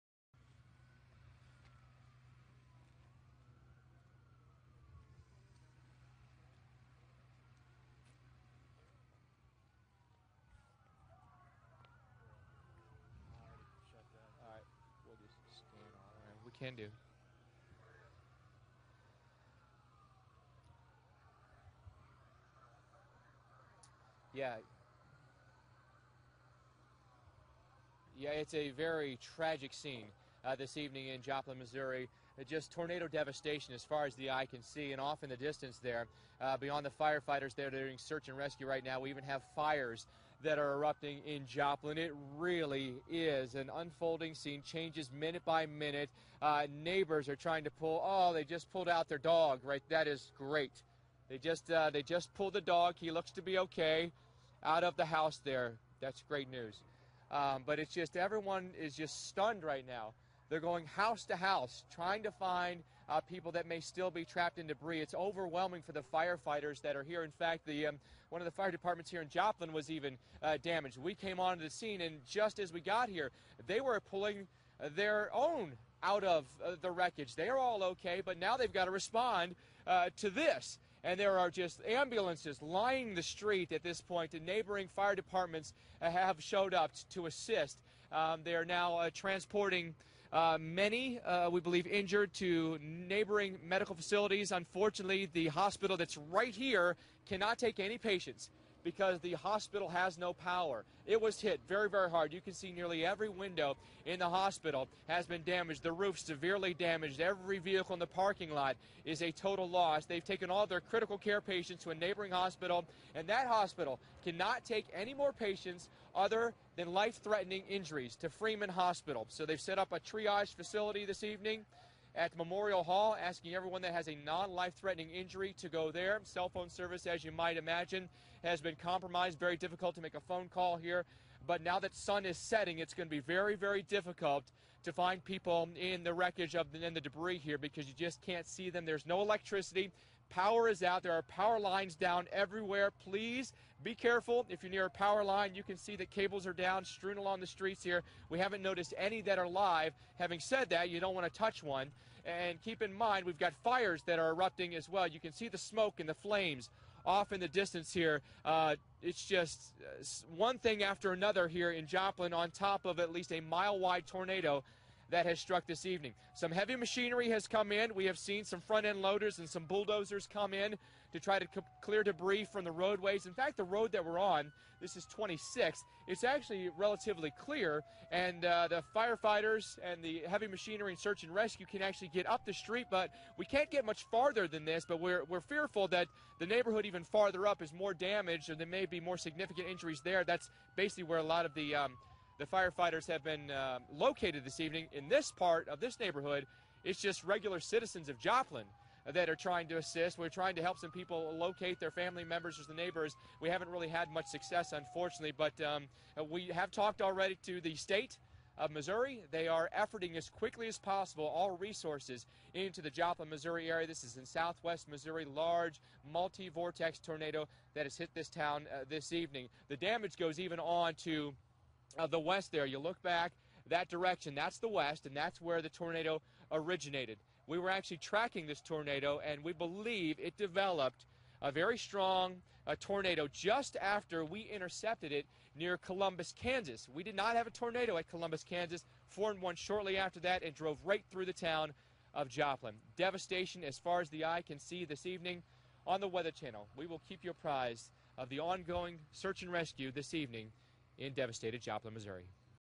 The First Live Report After Joplin Tornado
This is one of Mike Bettes' first live reports from Joplin, MO just moments after a violent tornado struck the southwest Missouri city.